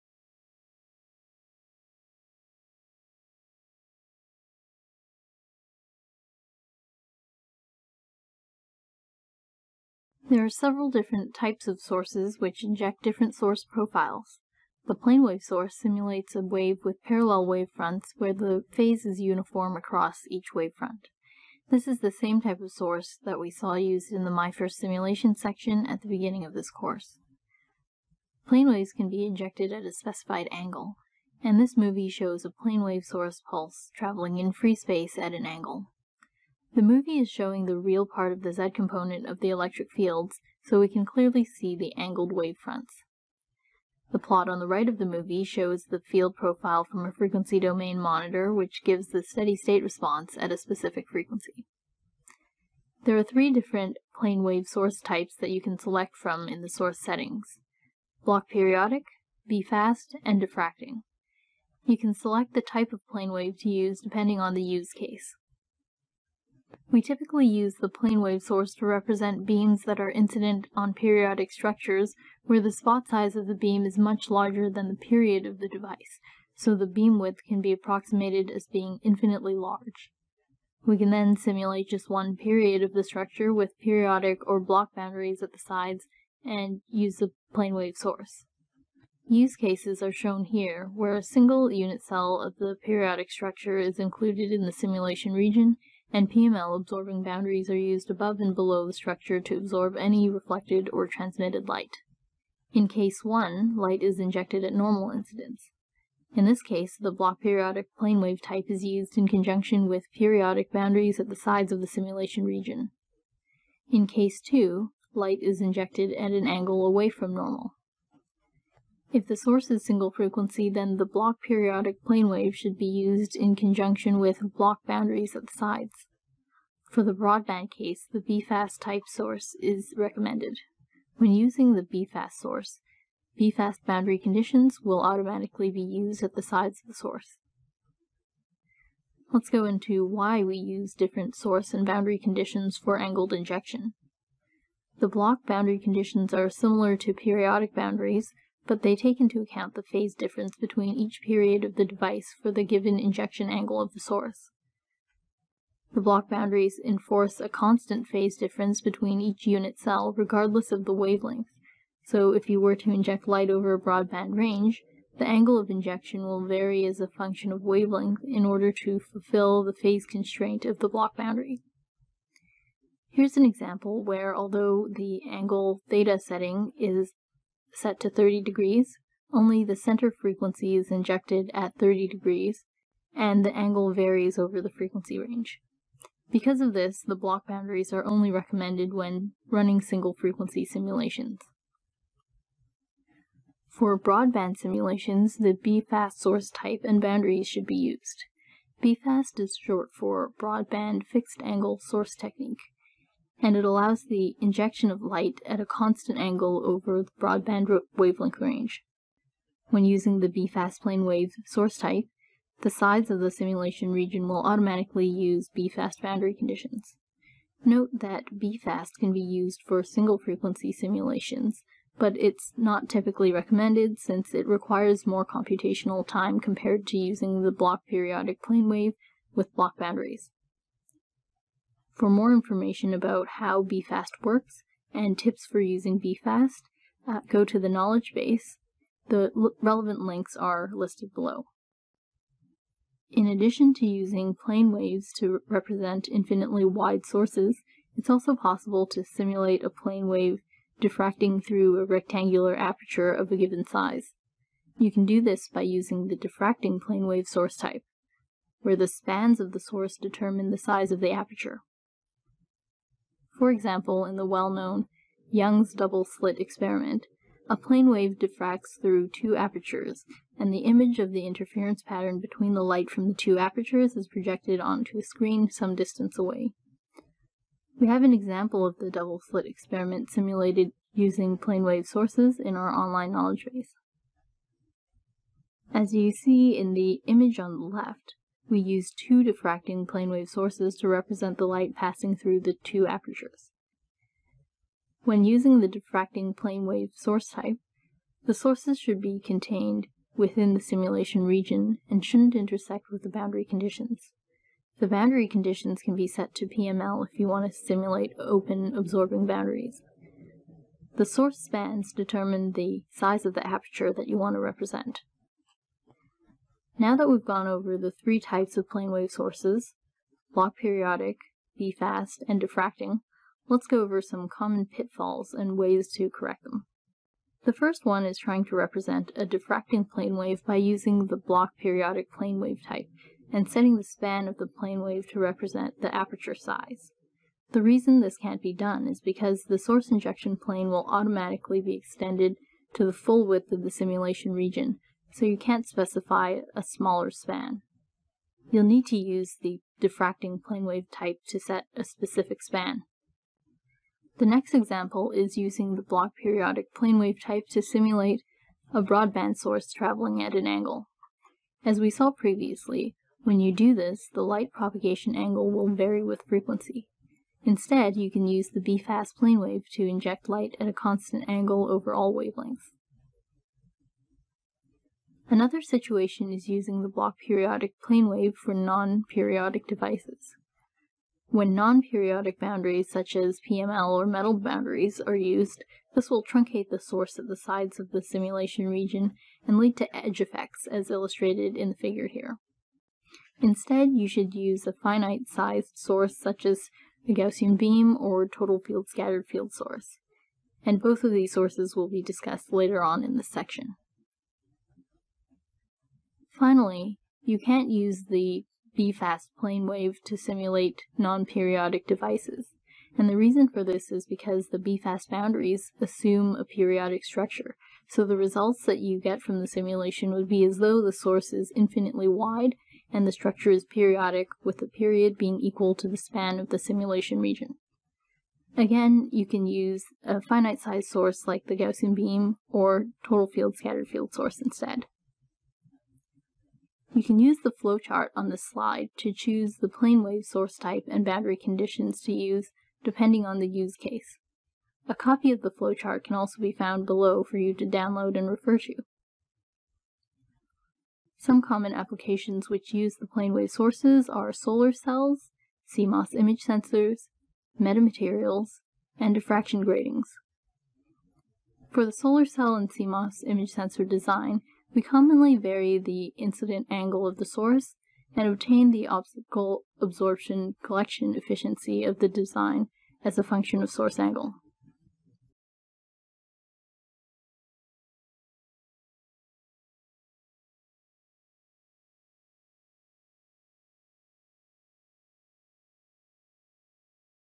Plane Wave Source in Ansys Lumerical FDTD – Lesson 2, Part 1
A plane wave is generally used together with a periodic structure. In this video lesson, you will three different types of plane wave sources: Bloch/periodic, BFAST and diffracting. This lesson is part of the Ansys Innovation Course: Ansys Lumerical FDTD – Sources. To access this and all of our free, online courses — featuring additional videos, quizzes and handouts — visit Ansys Innovation Courses